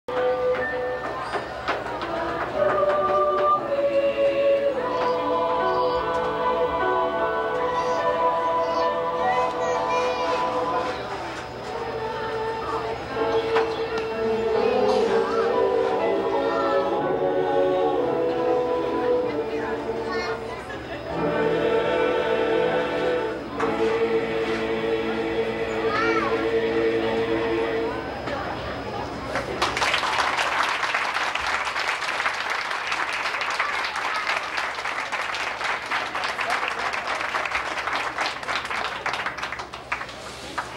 Beddingfield HS Vocal Ensemble
A very short clip of the award winning Vocal Ensemble from 1988 (I think) at the Wilson County Fair.  Mr. Ezzard directing!  I am standing on the top riser on the far right!  (ironic, now I am on the far left!)